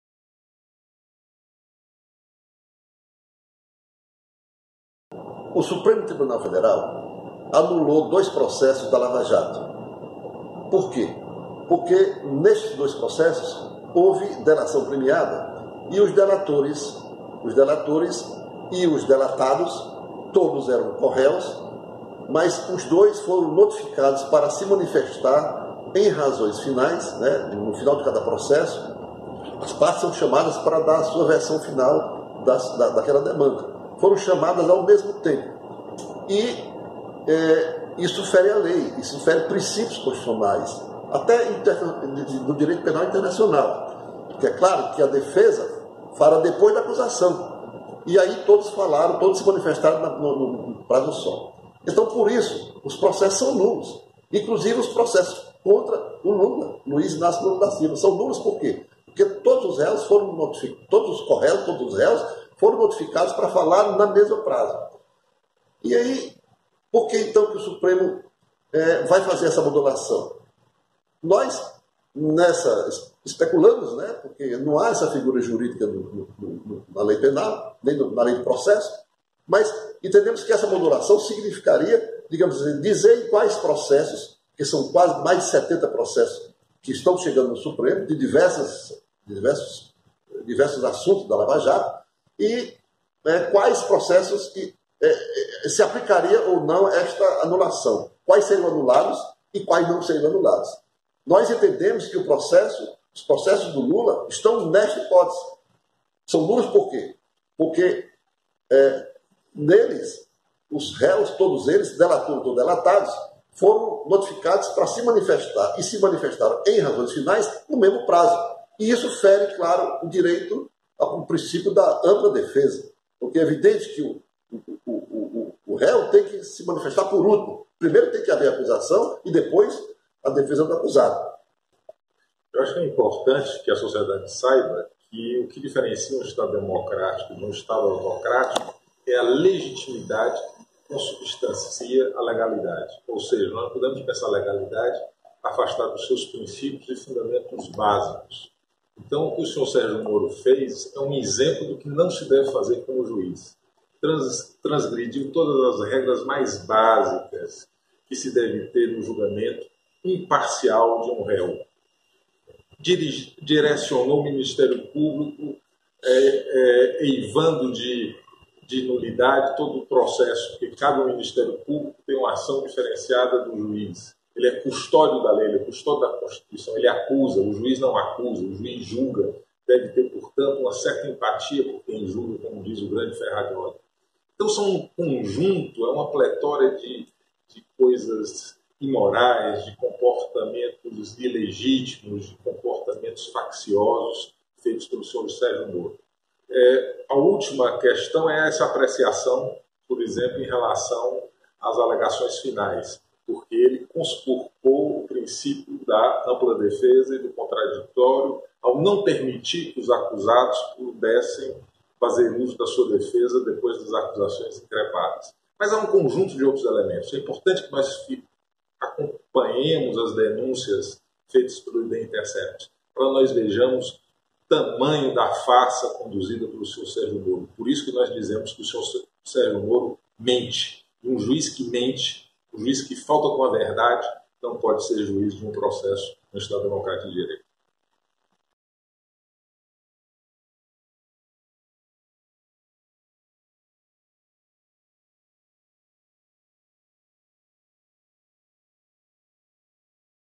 Por que o presidente do STF optou fazer uma modulação da Lava Jato?
Por que o presidente do STF optou fazer uma modulação da anulação das decisões dos processos da Lava Jato?